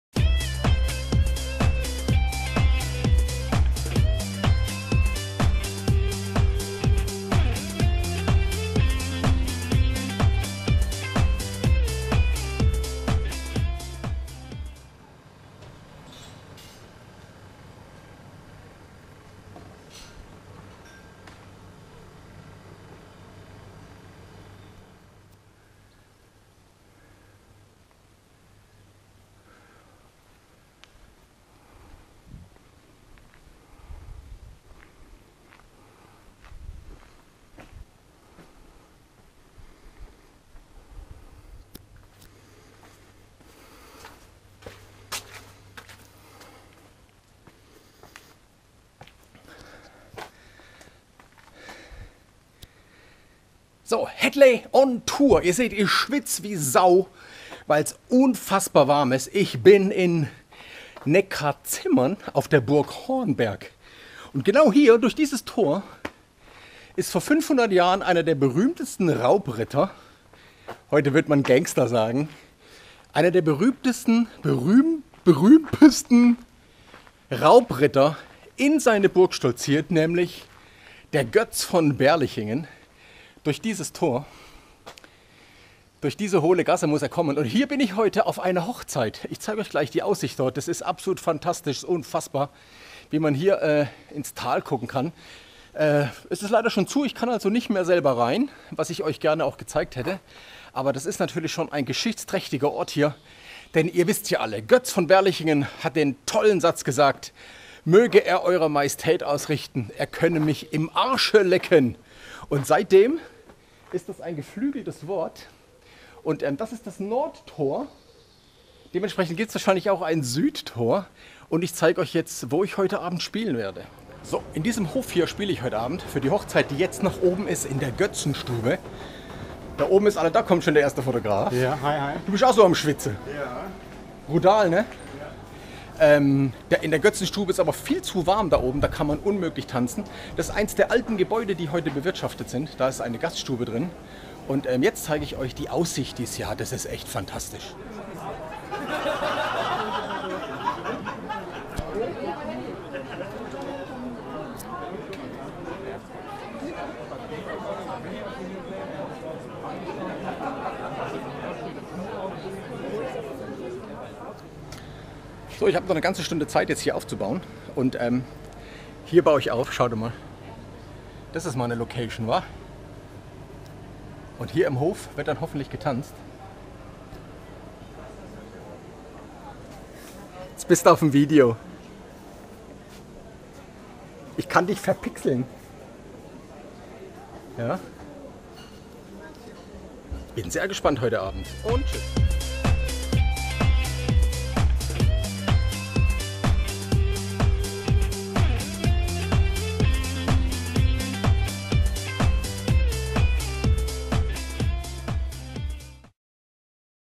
Hadley on Tour in Neckarzimmern....
... auf der Burg Hornberg. Ein unfassbare Hitze war da oben, selbst um 1 Uhr hatten wir im Burghof noch 28 Grad!